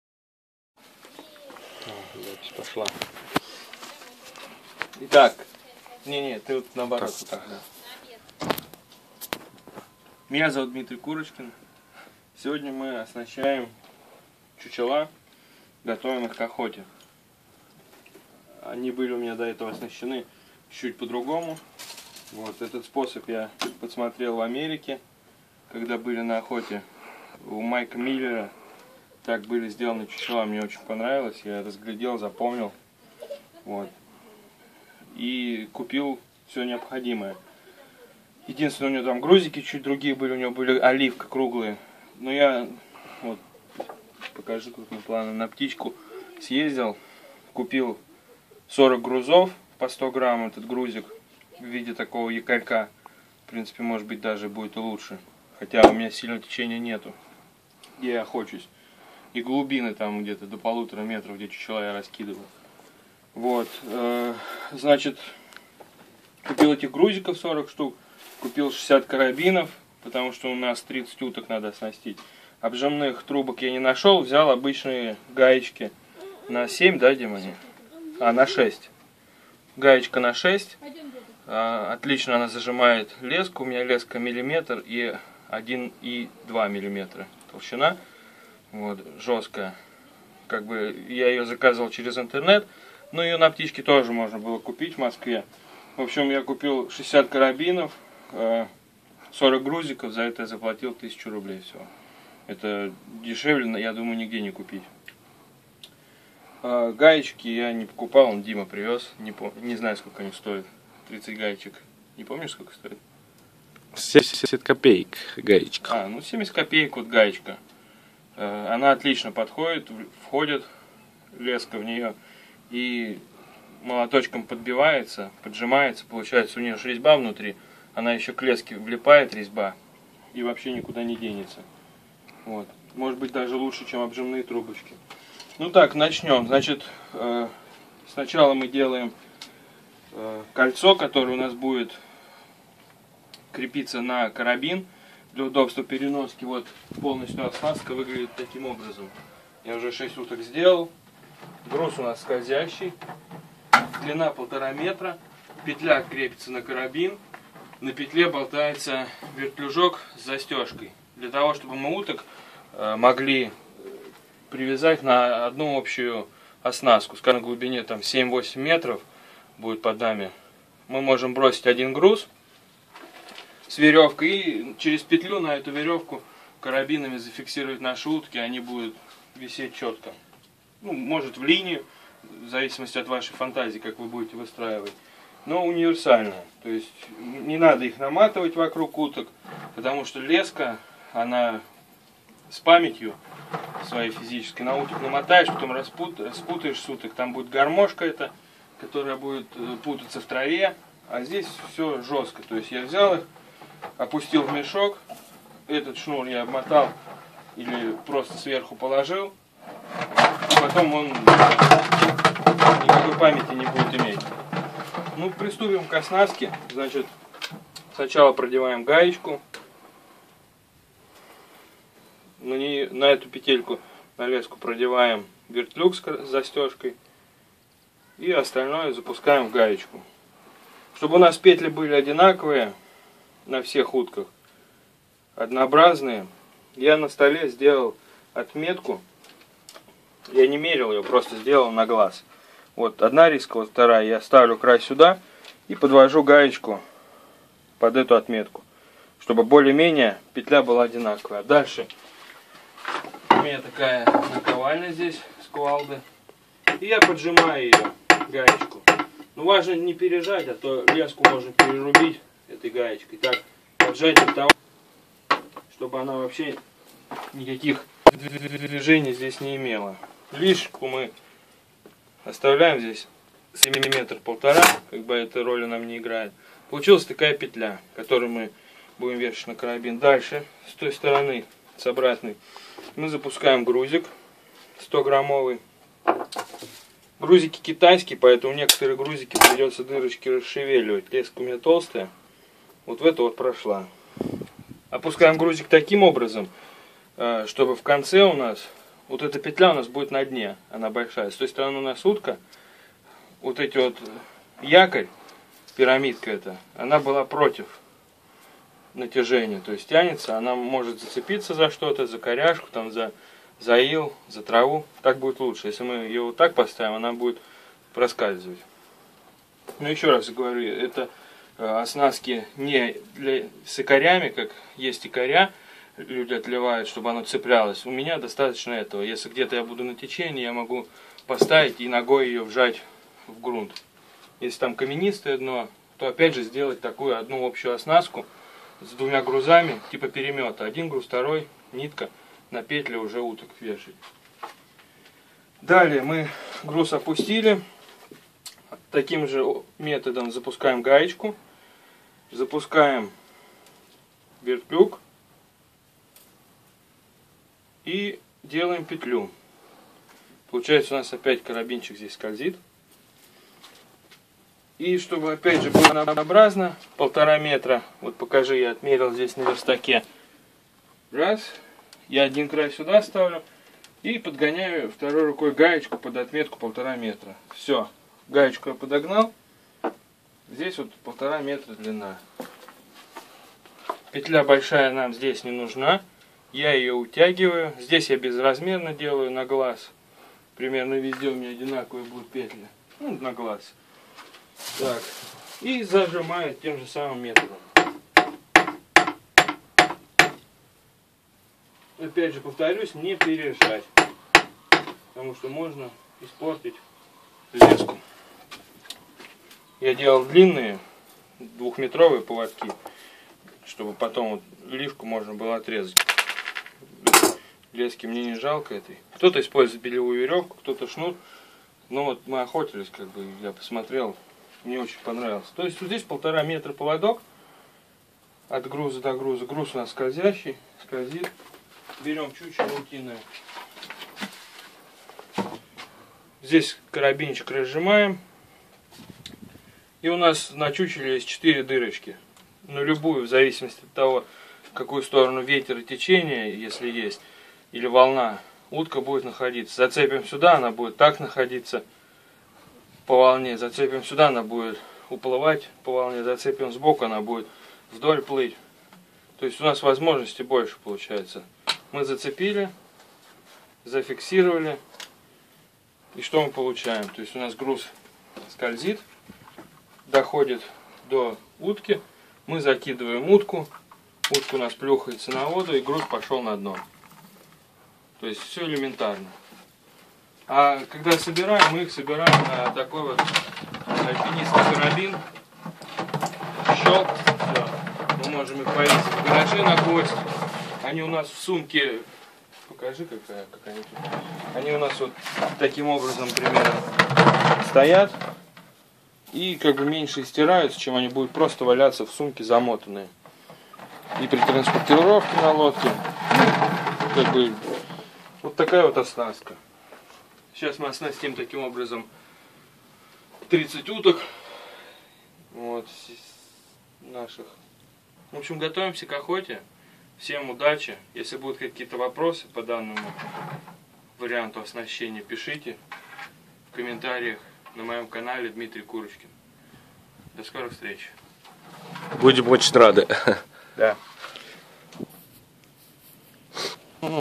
Оснастка для утиных чучел. "Техасская связка"
Подготавливаем утиные чучела к охоте.
Для оснастки одного чучела со скользящим грузом на 1,5 метровом поводке нам потребуется:
1,80-2,0 метра лески, сечением 1,0-1,2 мм.
гаечка на 6 мм. - 2 штуки.
Два больших карабина с вертлюгом.
Груз 100 гр. (оливка или любой другой с продольным отверстием) 1 штука.
Инструмент: молоток, наковальня, ножницы.
Далее это количество умножается на количество чучел.
Карабин на большой петле нужен для крепления оснастки на петлю единого шнура, на котором чучела будут стоять в одну линию, или же в несколько линий, как будет угодно охотнику, где глубина больше 1,5 метра. Опускается груз на дно (пакет с песком, кирпич и т.д.). Как груз достиг дна вяжется поплавок и от него (поплавка) идет шнур с петлями за которые и крепятся застежками (на большой петле) чучела в одну линию (через одинаковые или разные расстояния) на конце точно также, вяжется поплавок и опускается груз на дно. Такую оснастку можно сделать за ранее и хранить на мотовиле, для непредвиденных охот на большой глубине. Также ее можно сделать в походных условиях...было бы желание и веревка.